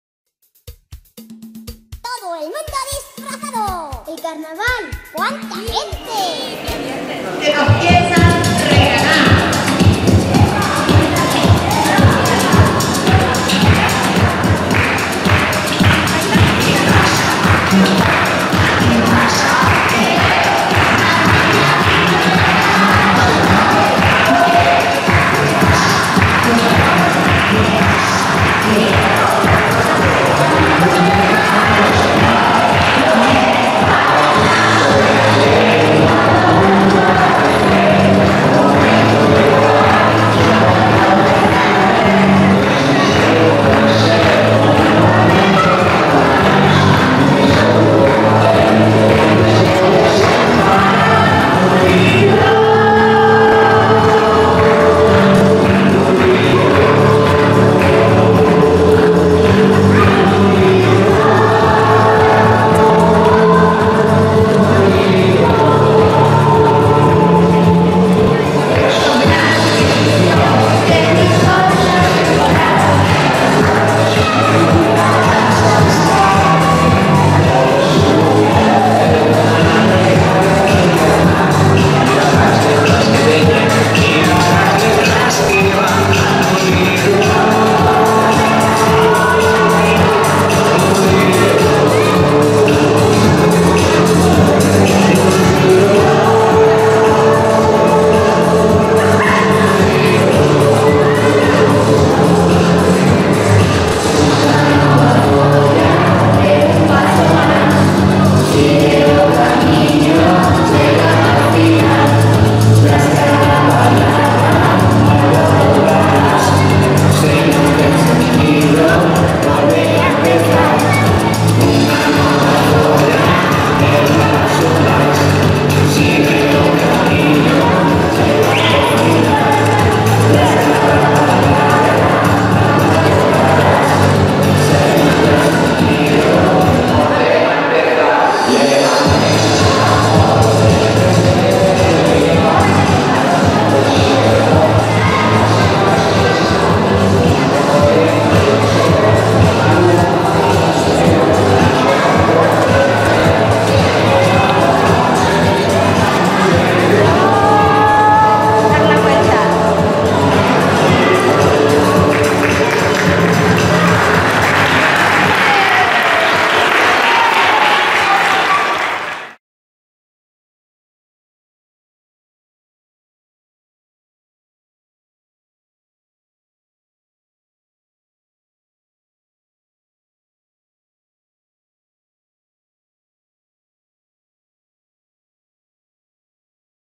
Hans Christian Andersen
Disfraces do alumnado de 4º de Ed. Primaria sobre os contos de H. C. Andersen